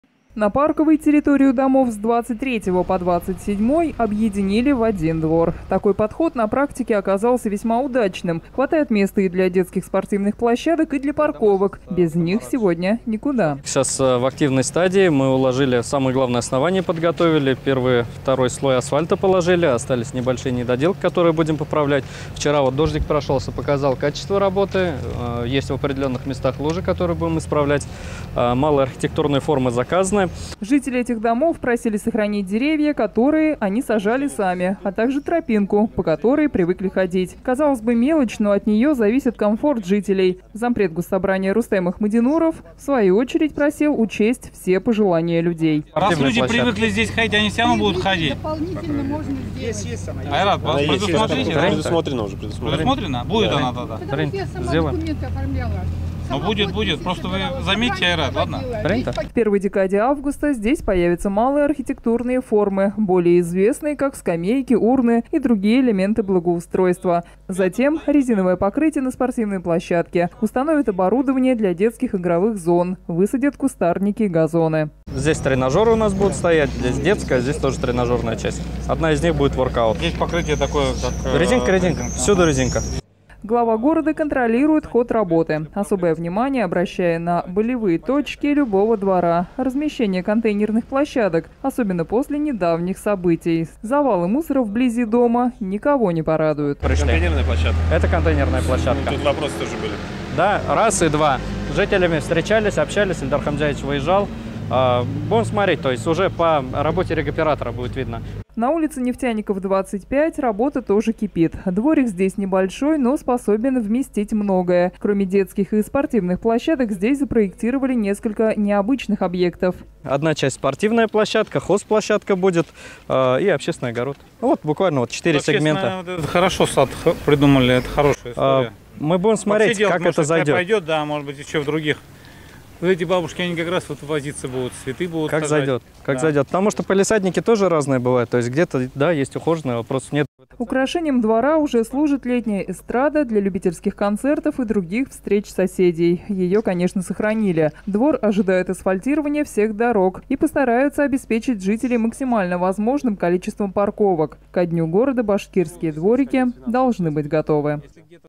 Ремонт дворов продолжается
«Башкирские дворики» продолжают создавать в двух кварталах города: на улицах Парковой и Нефтяников. Проекты планировки этих территорий не будут типовыми. В каждом дворе – есть свои особенности, сложившиеся за десятилетия. Их постарались учесть. Объекты посетил зампред Госсобрания – Курултая Рустем Ахмадинуров